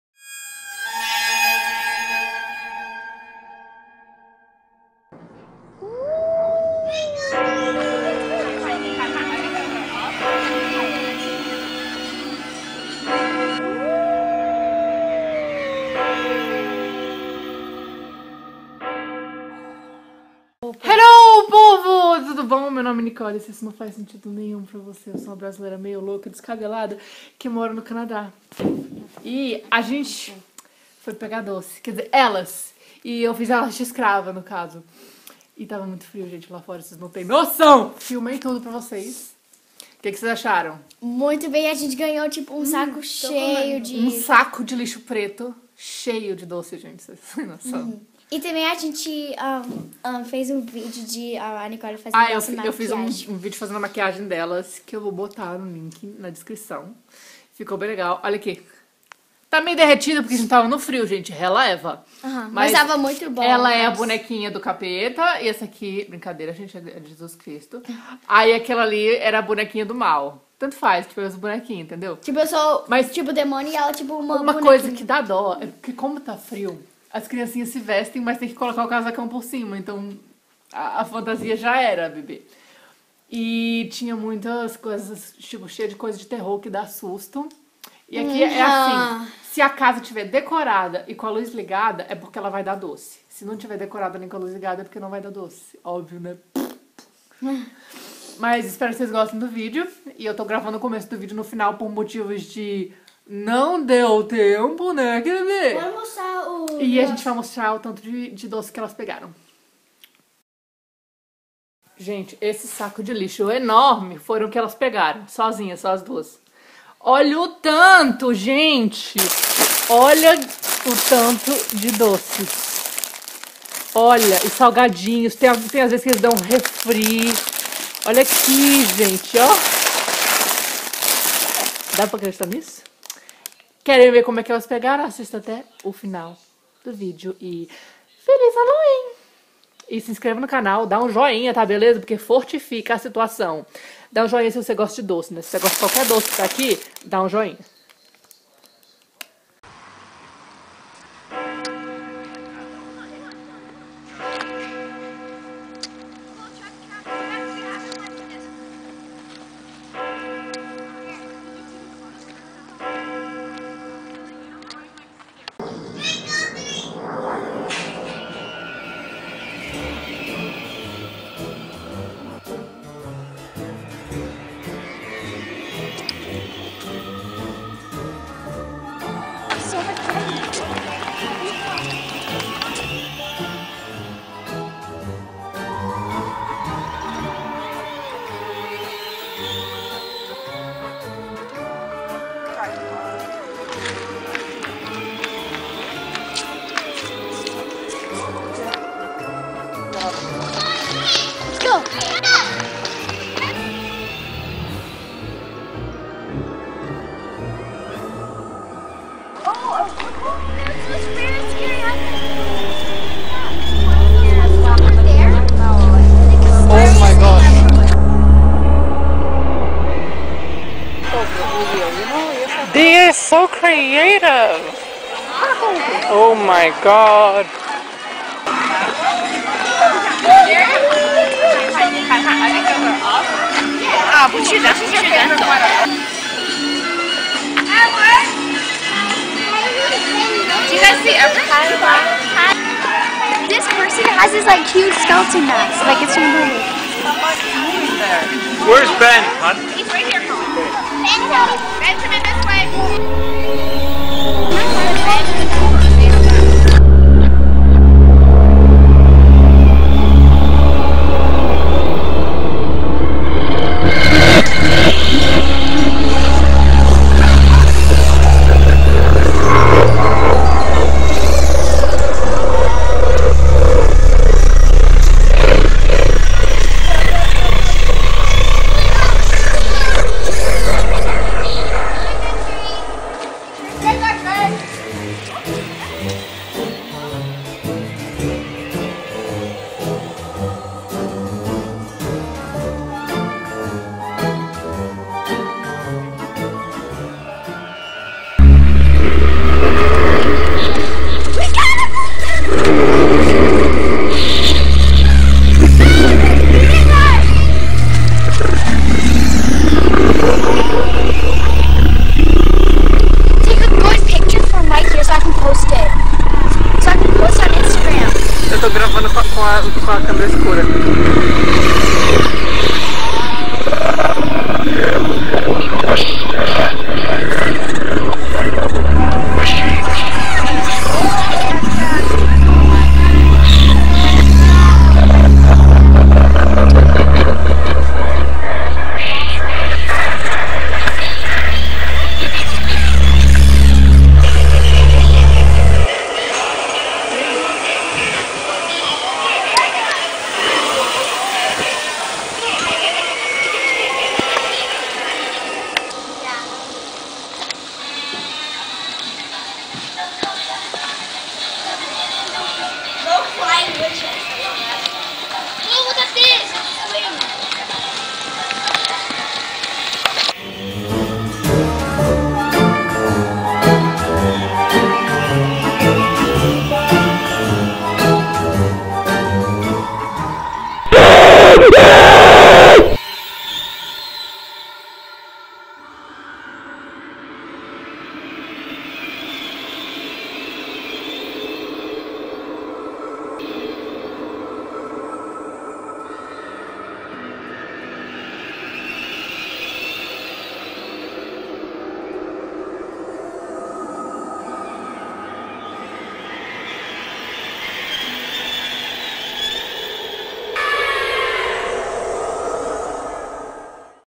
HALLOWEEN NO CANADÁ | Pegando doces
Nesse vídeo eu vou mostrar para vocês como é o Halloween no Canadá

Meu nome é Nicole Regiane (Nica) e aqui no canal Nica por aí você encontra curiosidades sobre o Canadá, viagens, receitas, vlogs, vida real e muito mais.